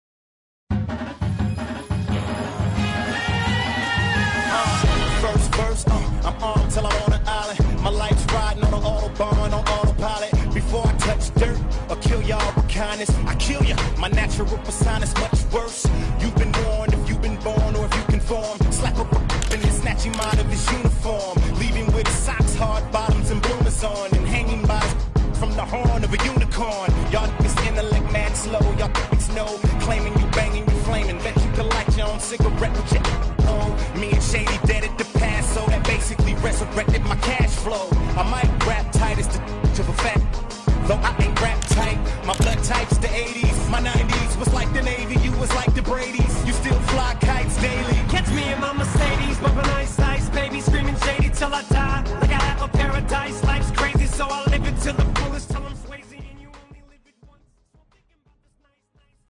Lookah Hookah Fun
Me and Friends having fun and hanging out at Lookah Hookah
585292LOOK Rochester Ny